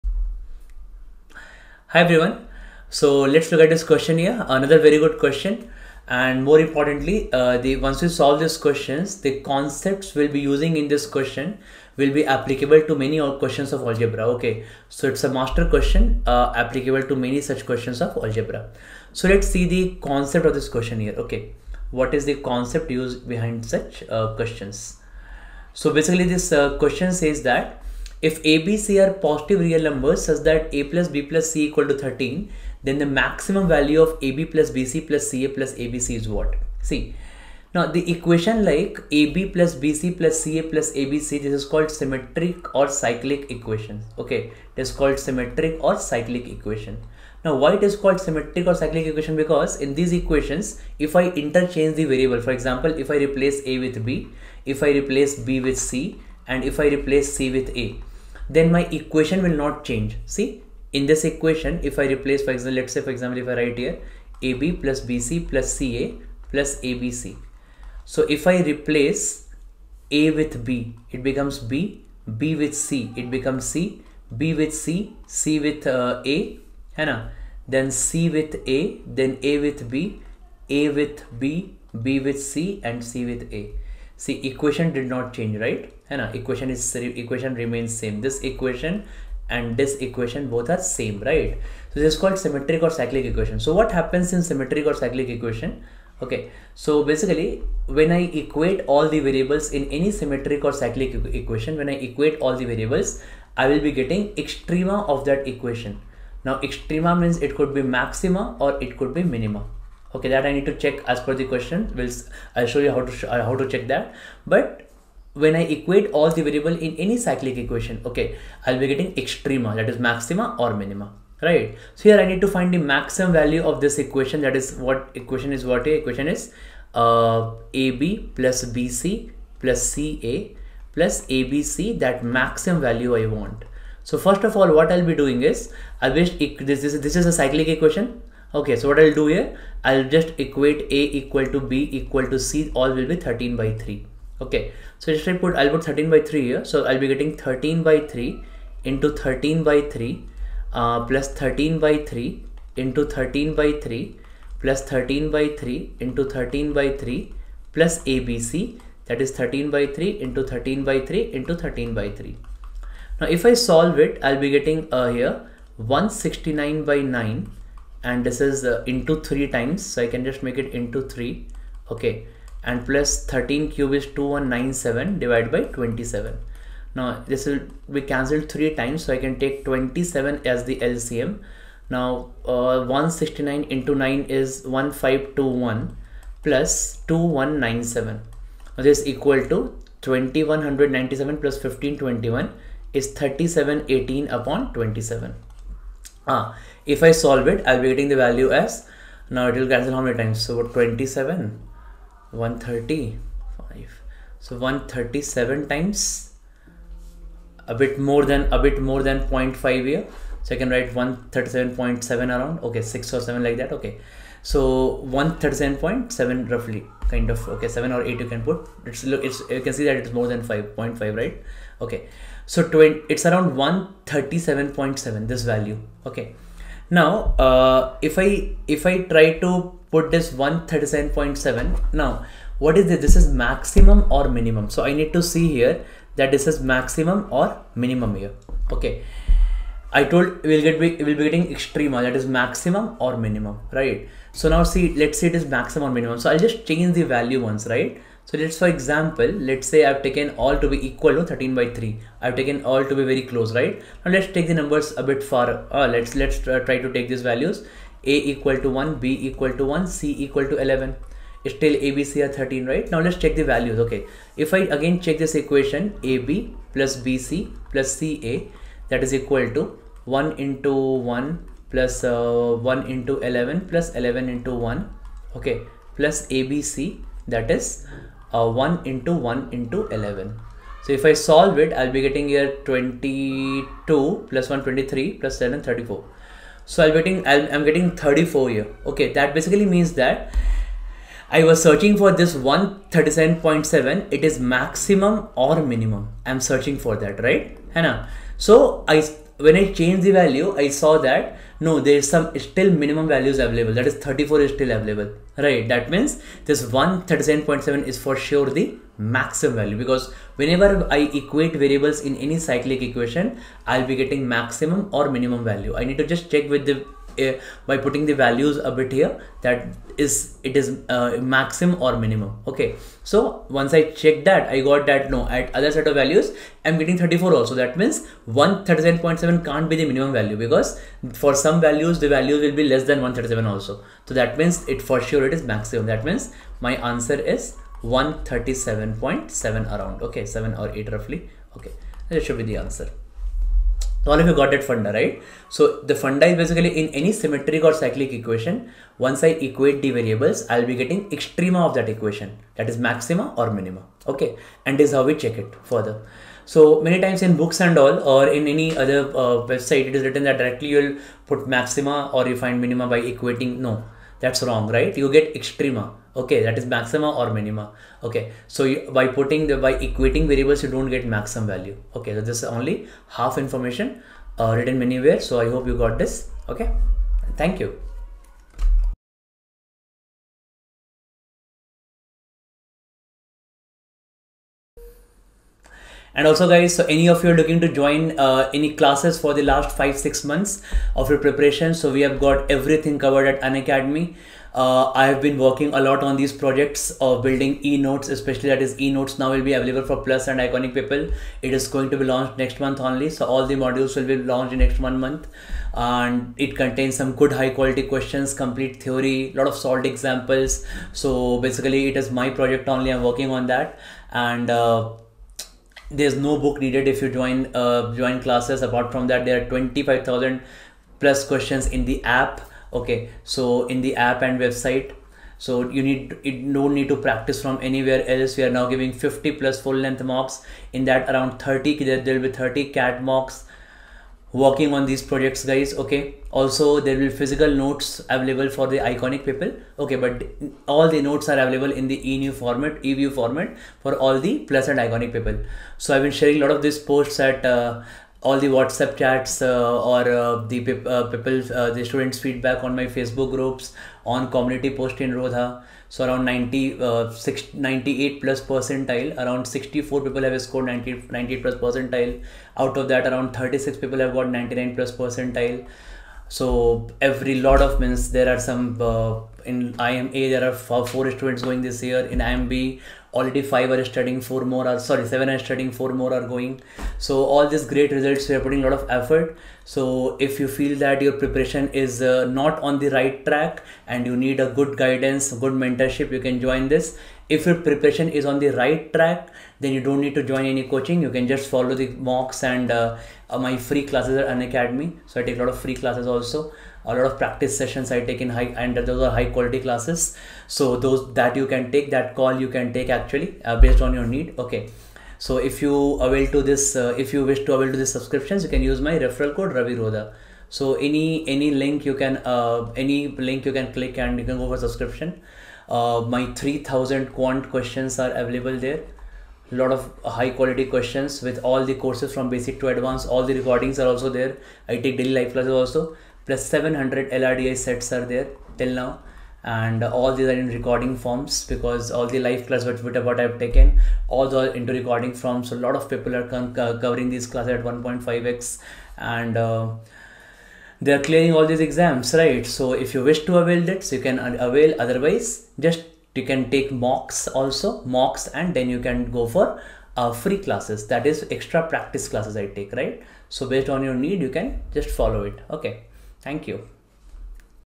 Advance Level Practice Questions of Algebra I Symmetry I Quantitative Aptitude for CAT
This practice session continues Inequalities in CAT Quant, focusing on tougher problems and CAT-level applications. Ravi Prakash Sir ensures you learn time-saving strategies. Great for CAT 2025 and CAT 2026 aspirants.

Enroll in Rodha's Full CAT 2025 Course (with Dedicated Doubt Support & Structured Plans)

CAT2025